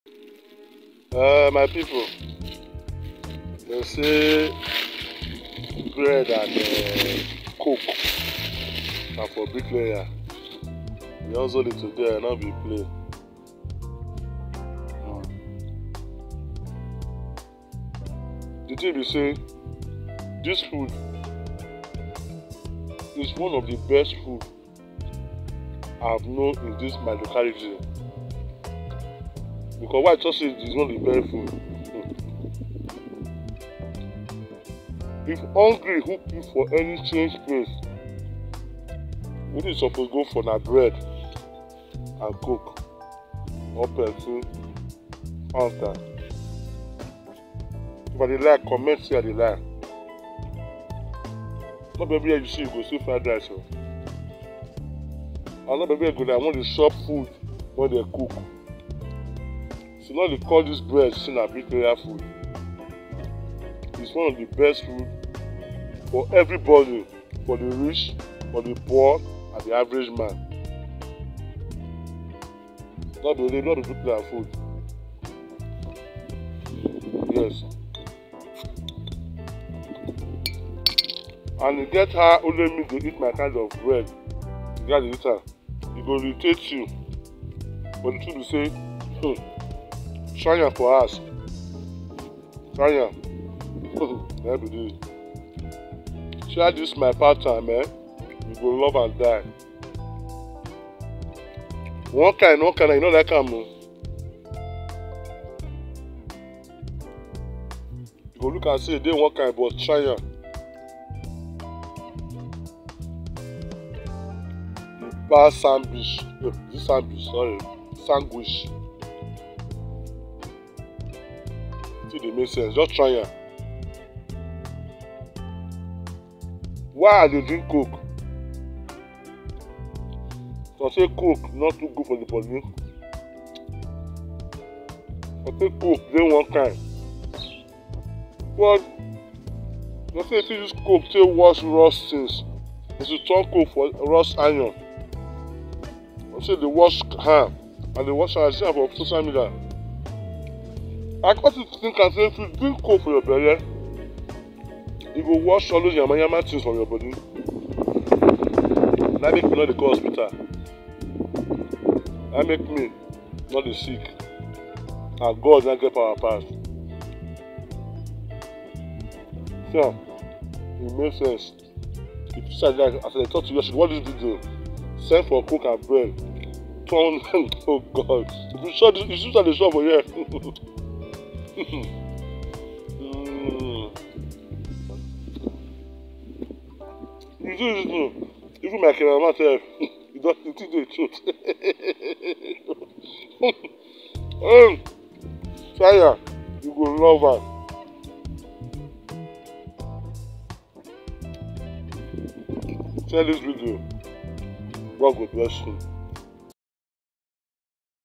bricklayer food
talking about Coke and bread